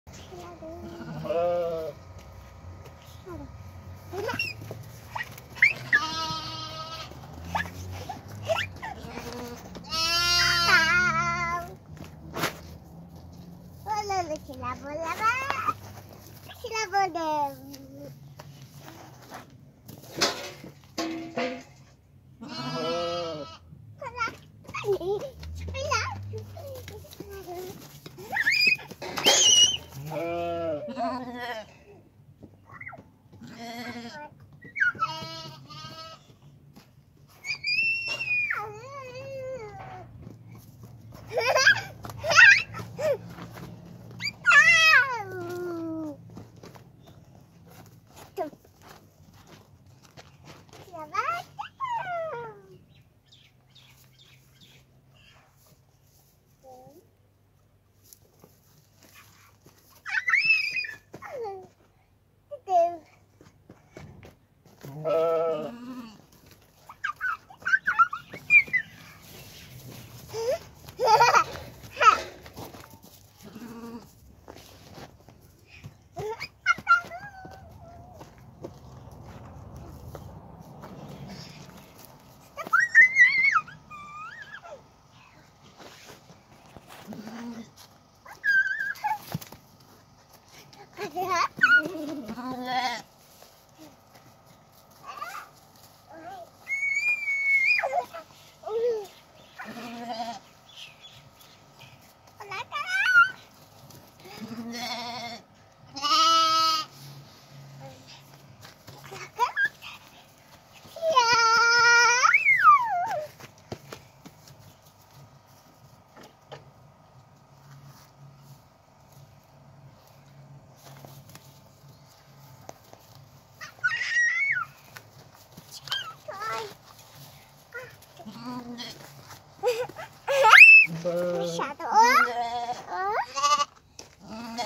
fermer baby 3 Uncovering the Playful Delights
Join us on this snowy adventure as we continue to uncover the playful delights of winter! Part 2 of our series will take you through more snowy activities and fun, including sledding, snowball fights, and building a snowman. Don't miss out on the winter wonderland, watch now!
Hello friends!